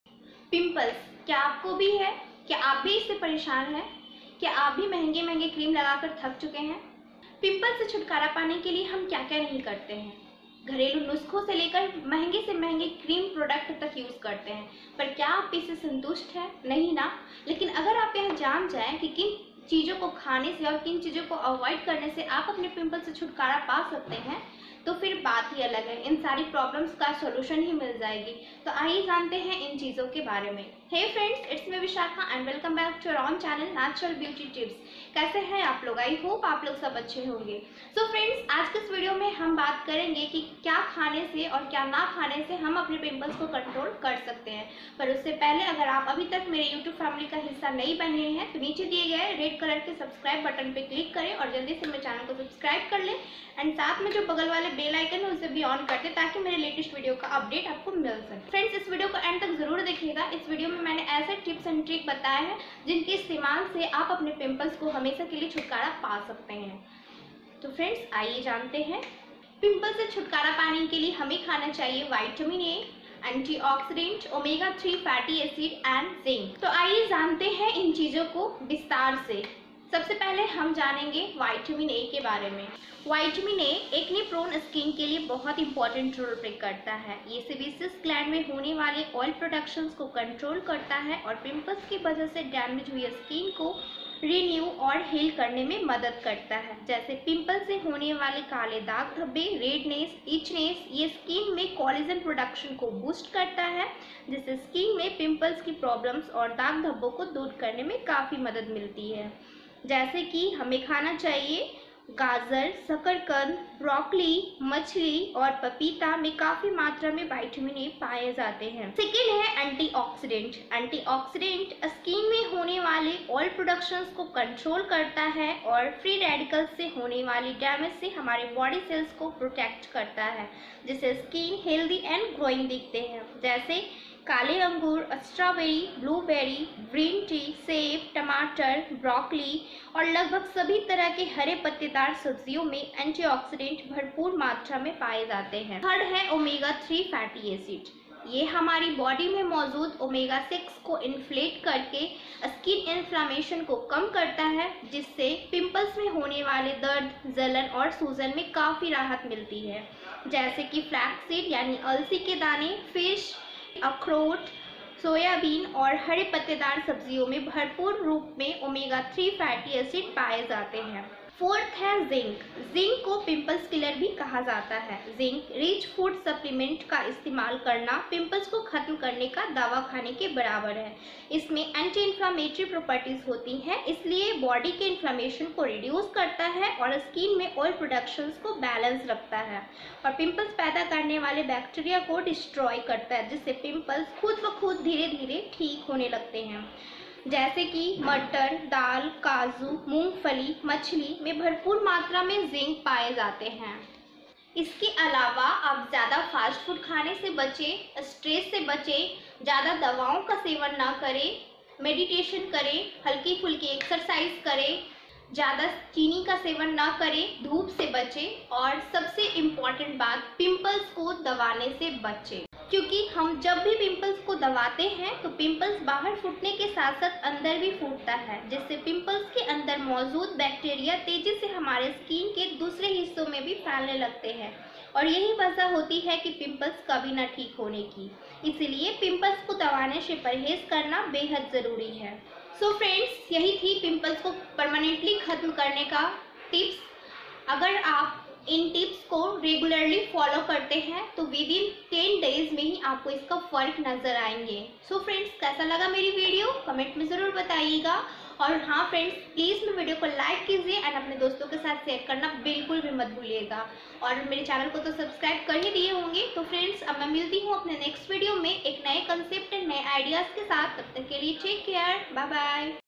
How To Remove Pimples | पिम्पल्स होने पर इन चीजों को खाकर हमेशा के लिए ठीक करें
आज के इस  वीडियो में हम जानेंगे की कैसे हम अपनी चेहरे से पिम्पल्स को खत्म कैसे करें और वो भी बिना पैसे खर्च किये हुए नैचुरली।